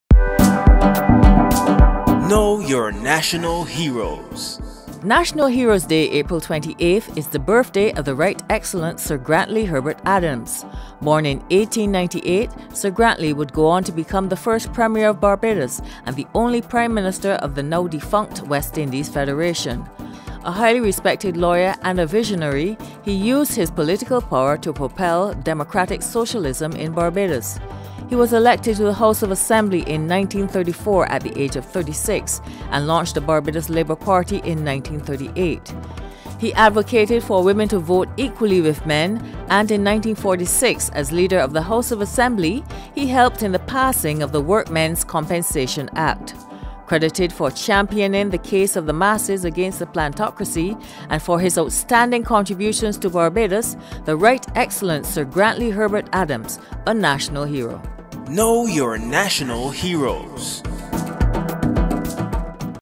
National Heroes Promo - The Right Excellent Sir Grantley Adams
National Heroes' Day is celebrated in Barbados on April 28.

Learn more about the life of Barbados' first Premier and only Prime Minister of the defunct West Indies Federation, The Right Excellent Sir Grantley Adams.